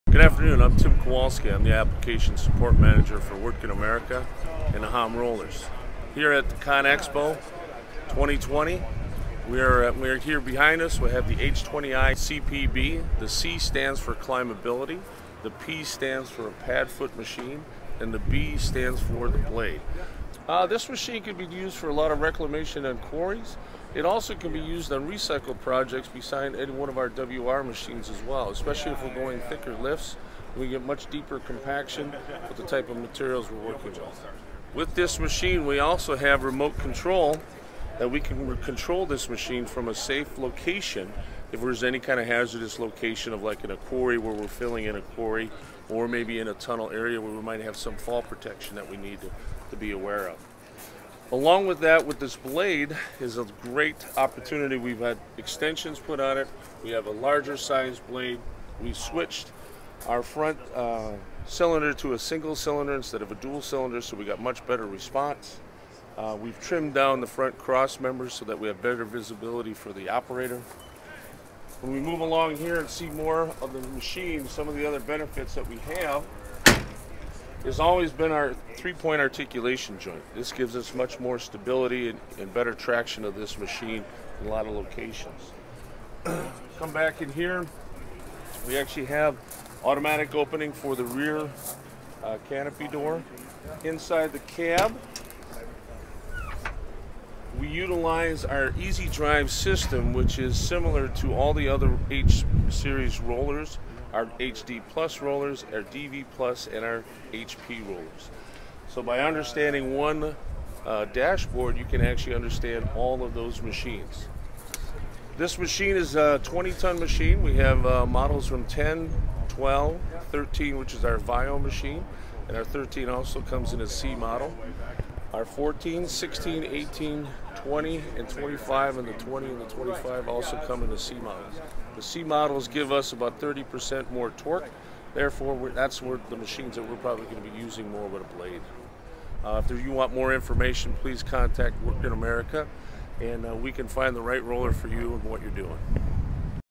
ConExpo 2020: Hamm’s Remote Control Roller
Hamm, a Wirtgen company, displayed its remote controlled roller at ConExpo 2020. Tim Kowalski walks us through its features.